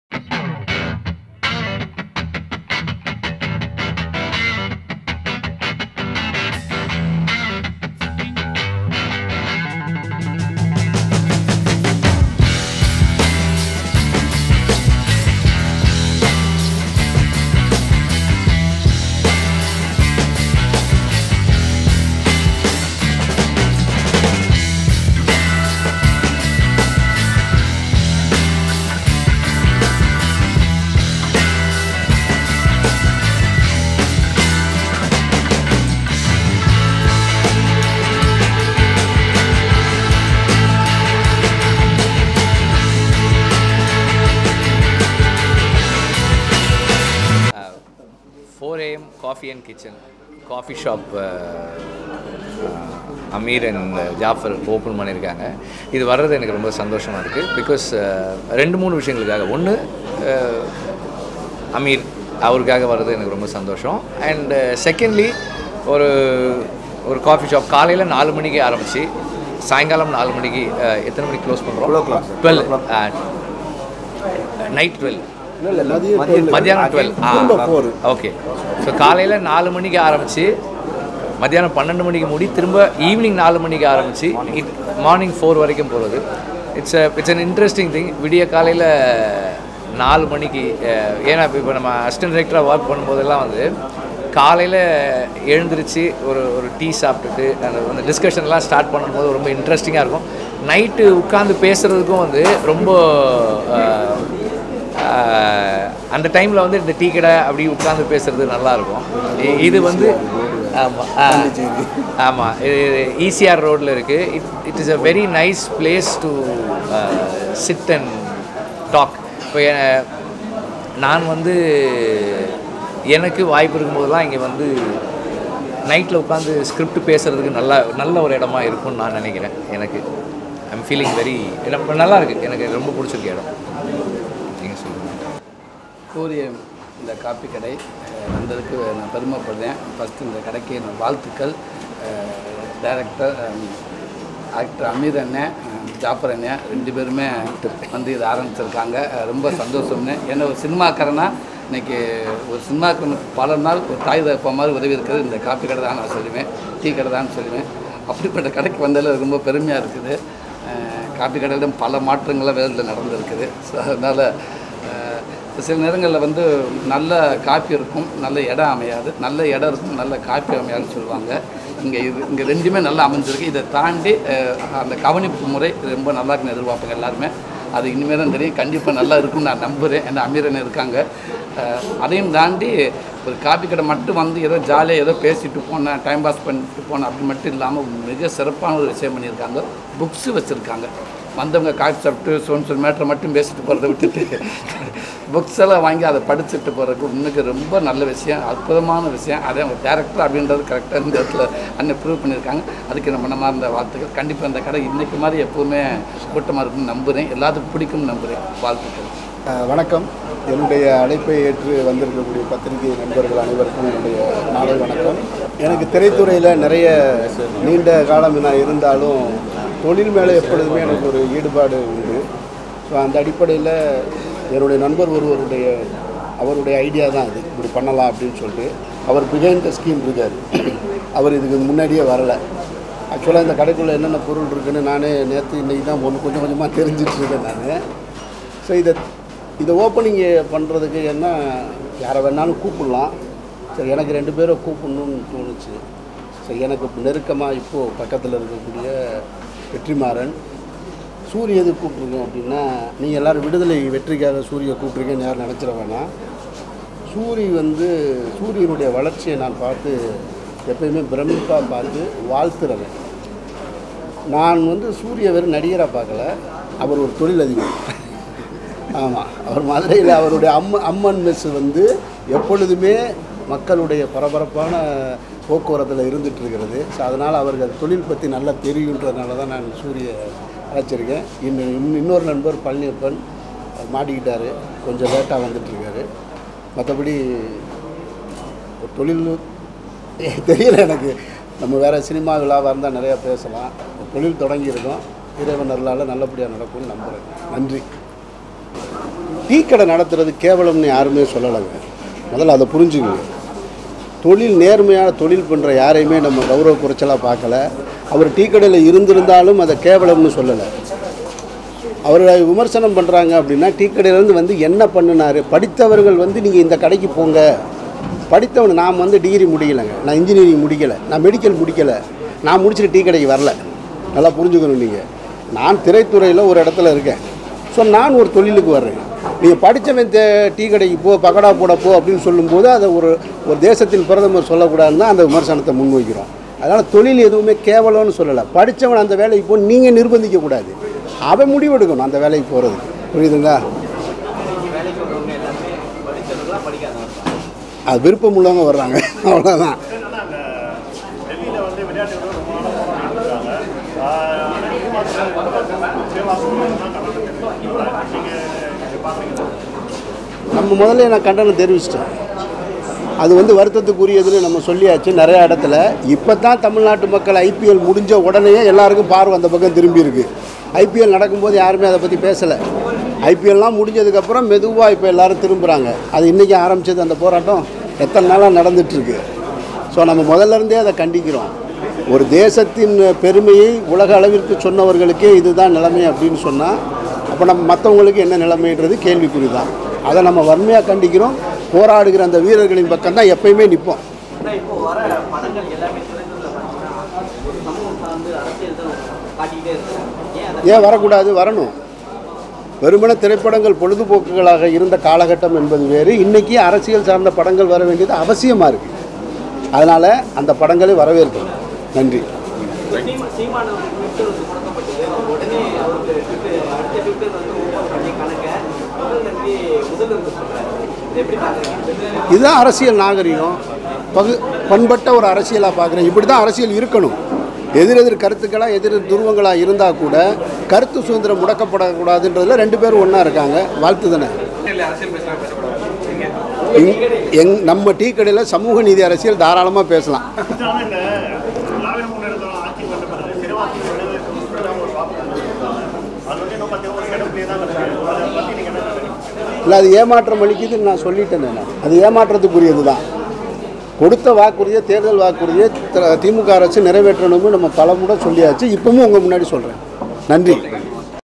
DirVetrimaaran and Actor soori inaugurated : Dir Ameer 'new restaurant 4am Cafe & Kitchen
: Director Vetrimaaran and Actor @soori inaugurated
[Director Ameer 's new restaurant 4am Cafe & Kitchen at ECR near Uthandi toll plaza and Mayajaal